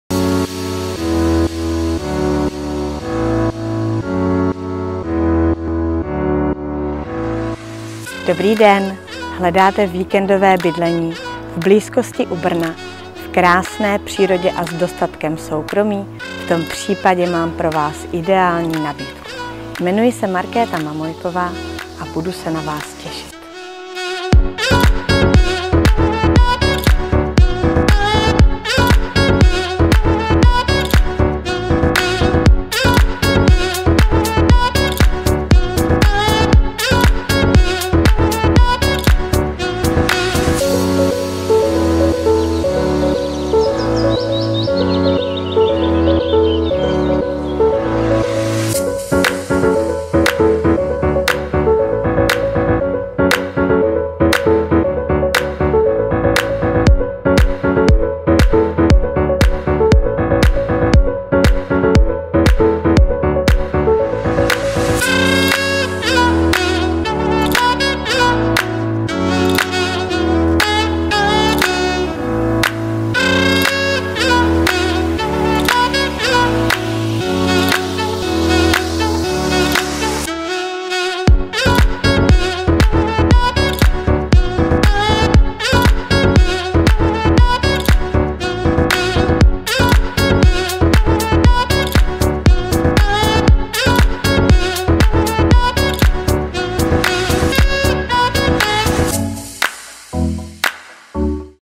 Video 205711 k nabídce ID26947
Hledáte místo k víkendovému odpočinku v přírodě, s dostatkem soukromí, a přitom v blízkosti Brna?  V tom případě mám pro Vás ideální řešení.  Pojďte se se mnou podívat na krásnou chatu v obci Židlochovice, která nabízí také možnost trvalého bydlení.
Chata stojí na pozemku o velikosti 3475 m2 a je dvoupatrová. Stavební materiál jsou tvárnice, strop desky Hurdis a horní patro je z části tvořeno dřevěnou konstrukcí. Zahradní dřevěná krytá terasa byla postavena v roce 2009. V přízemí domu se nachází vstupní chodba, kuchyň, obývací pokoj a WC. V patře je prostorná, klimatizovaná ložnice se vstupem na terasu a koupelna s rohovou vanou. Hlavní okna jsou nová plastová, malá okénka dřevěná. Celý dům je zateplen 10 cm tepelnou izolací a vytápěn přímotopy v kombinaci s kamny na dřevo. Teplou vodu zajišťuje elektrický ohřívač na 75 l. Městská voda je tekoucí po celý rok, a proto je možné celoroční využití objektu.
K dispozici je minimálně 5 parkovacích míst v dolní části zahrady, z toho jedno zastřešené. Za chatou se nachází odpočinková zóna zahrady s bazénem, ohništěm, grilem, posezením a zahradním domkem. Část zahrady je také připravena k drobnému pěstování zeleniny či bylinek. Umístění domu je na okraji krásného a plně občansky vybaveného Jihomoravského města Židlochovice. Má své číslo popisné, a proto je možné si zde zřídit trvalý pobyt.
Pomůžeme Vám také zajistit financování nemovitosti za nejvýhodnějších podmínek na trhu. Neváhejte nás kontaktovat pro více informací či osobní prohlídku.